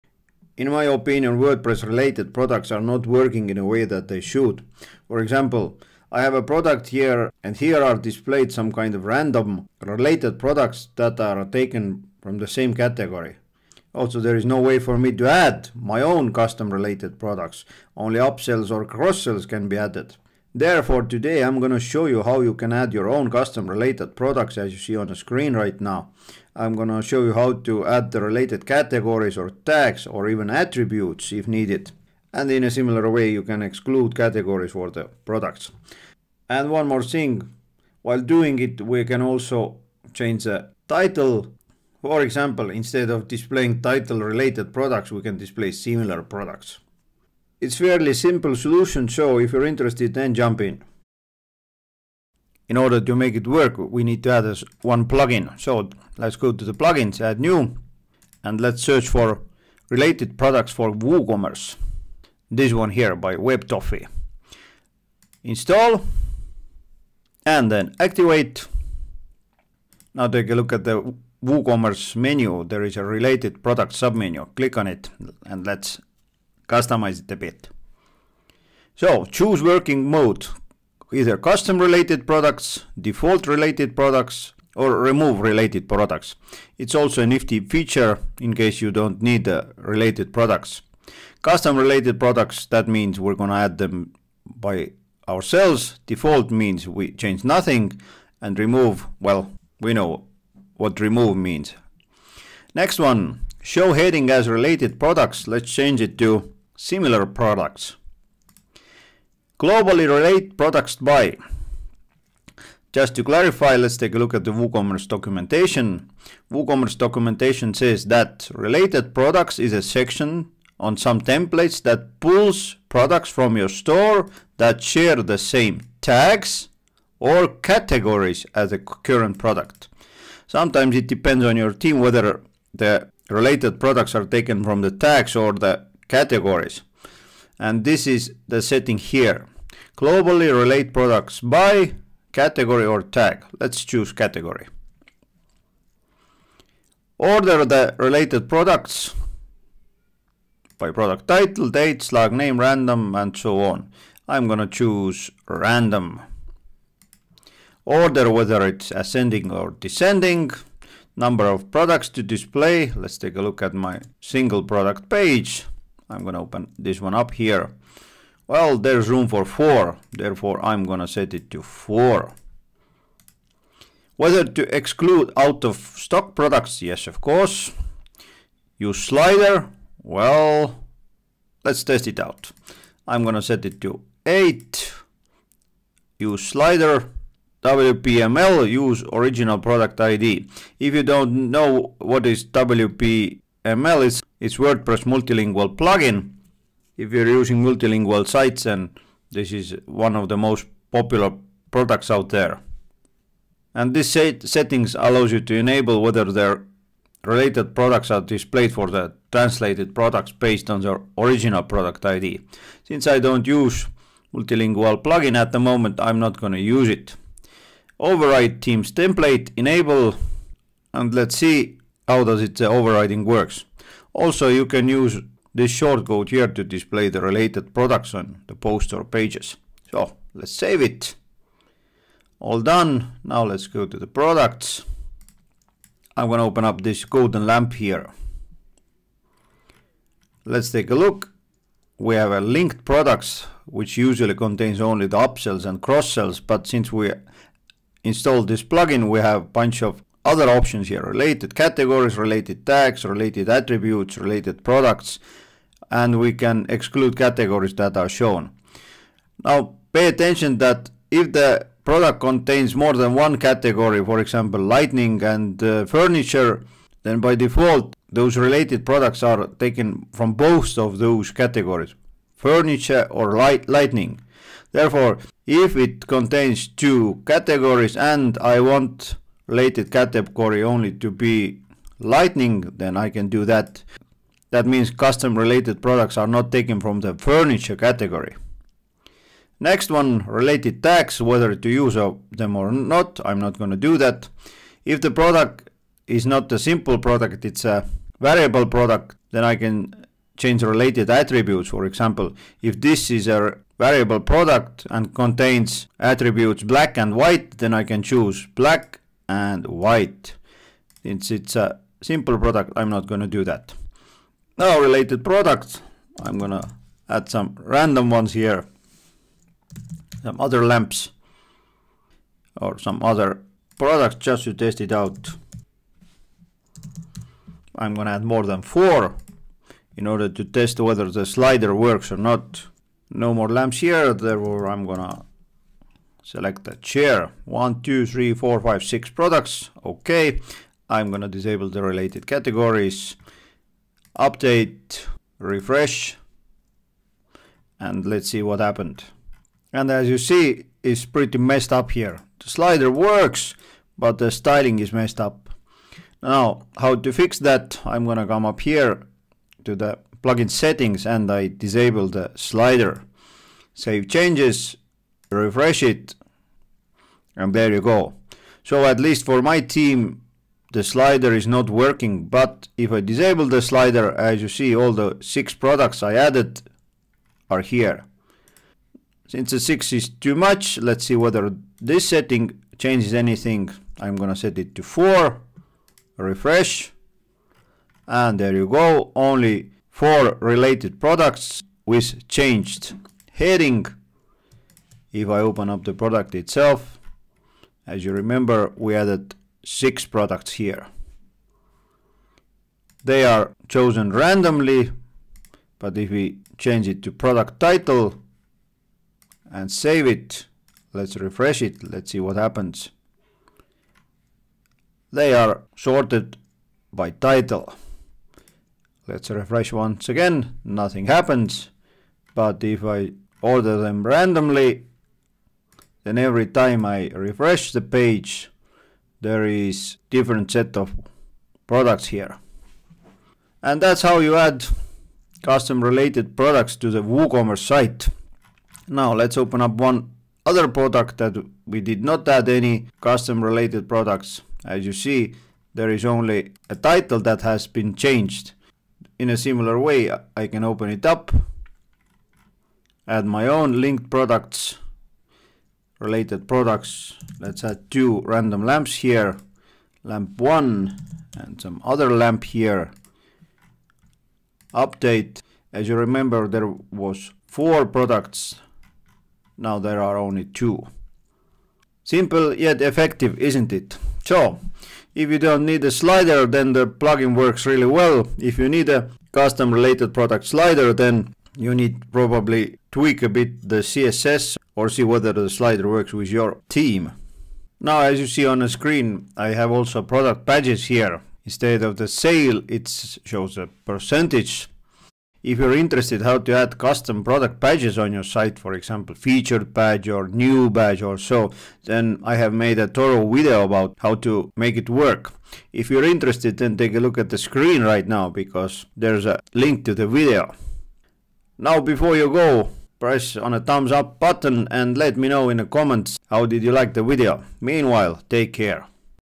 How to Set Up Woocommerce Custom Related Products? (3 minute hack)
In this video I’m going to teach you how to set up Woocommerce custom related products within a couple of minutes. Also, you will learn how to add related products by tags, related products by attributes and related products by category.

✅   AWESOME WOOOCOMMERCE EXTENSIONS:
Best Woocommerce Swiss Knife tool - Kadence WooExtras (SAVE 10% Coupon SIMPLEHACKS)